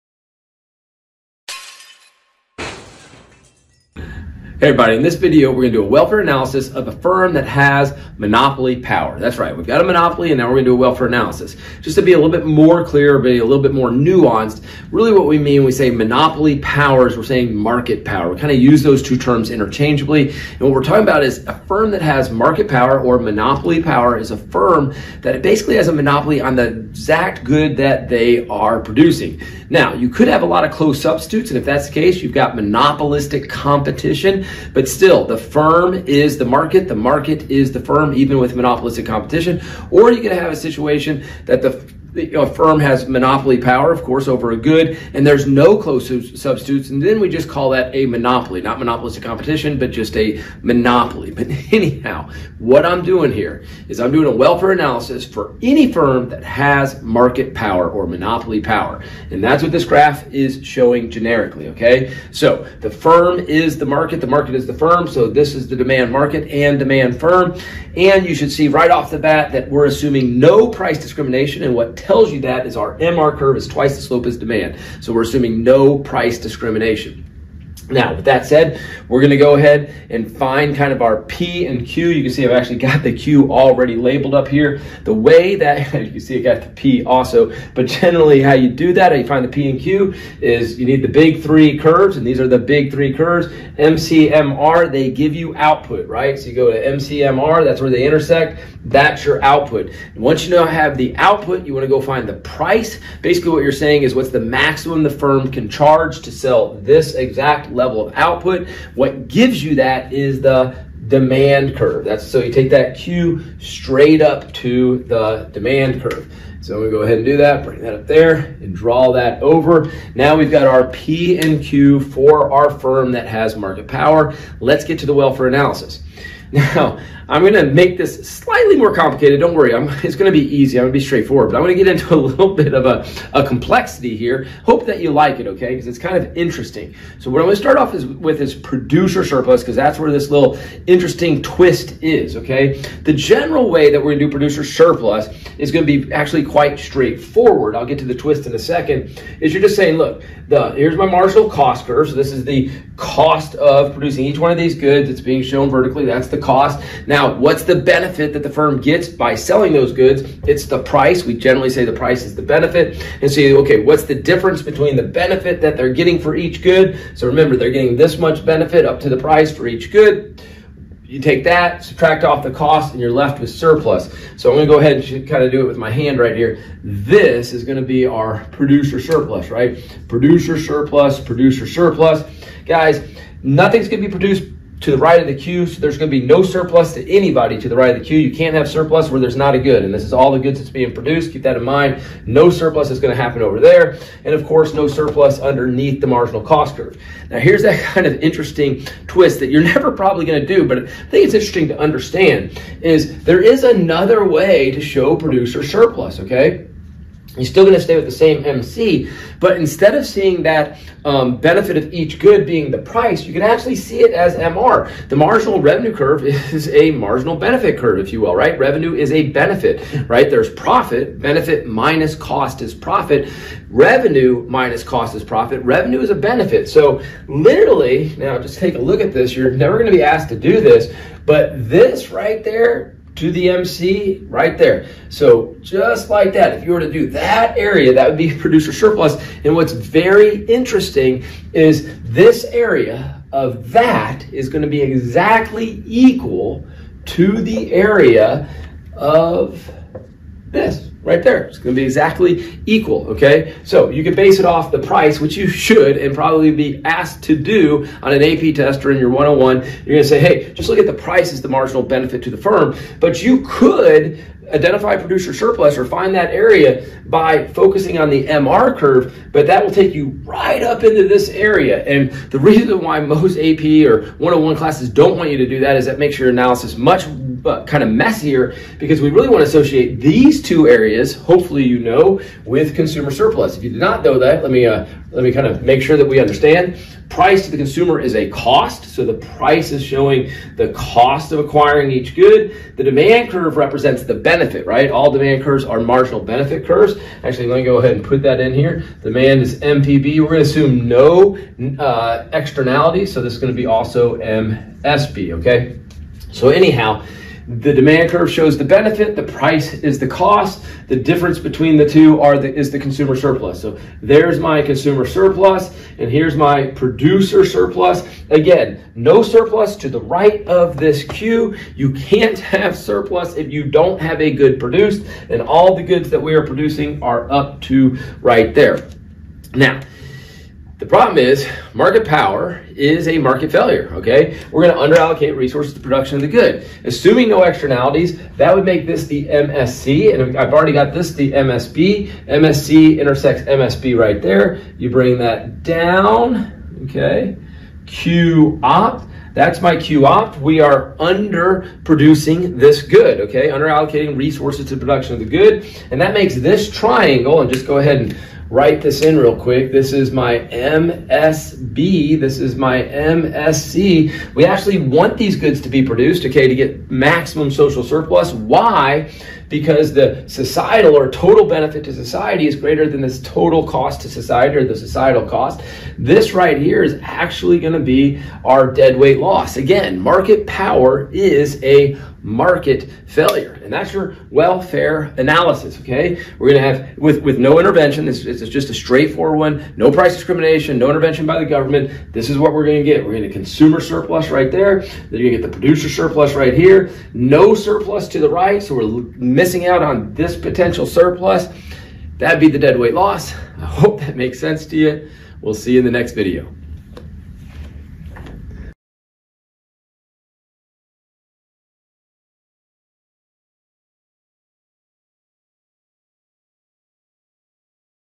A Welfare Analysis of A Firm with a Monopoly
Monopoly power, also called market power, results in dead-weight loss.  The power monopolist's have over price results in them producing less than the optimum quantity.  This video shows students how to find the consumer surplus, producer surplus, and deadweight loss associated with monopoly power.

This video is made for 1st year college students or AP/IB Economics students.  It focuses on foundational economic concepts.